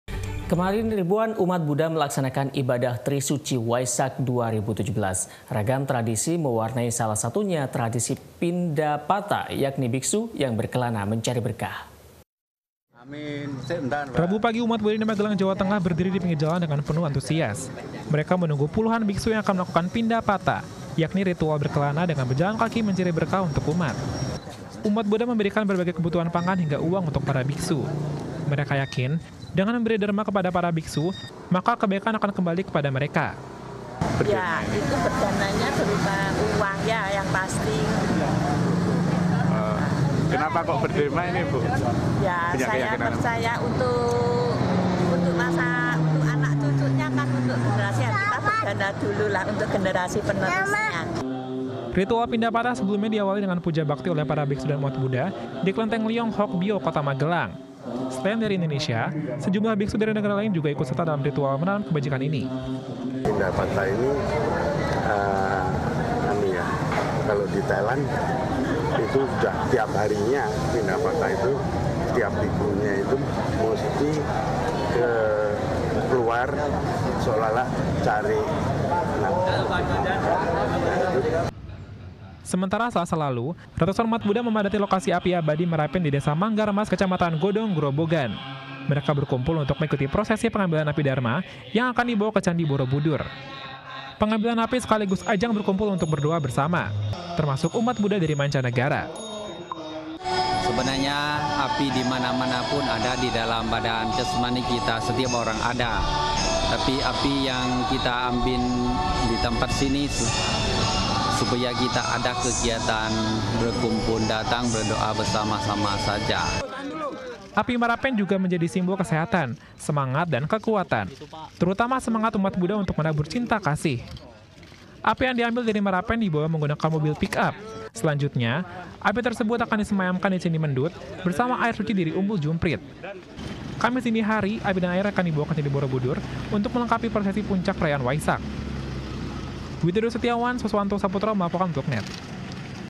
NET JATENG - TRI SUCI WAISAK 2017
Rabu pagi, umat budha di Magelang, Jawa Tengah, berdiri di pinggir jalan dengan penuh antusias. Mereka menunggu puluhan biksu yang akan melakukan pindapata. Yakni ritual berkelana dengan berjalan kaki mencari berkah untuk umat. Umat budha memberikan berbagai kebutuhan pangan hingga uang untuk para biksu. Mereka yakin, dengan memberi derma kepada para biksu maka kebaikan akan kembali pada mereka.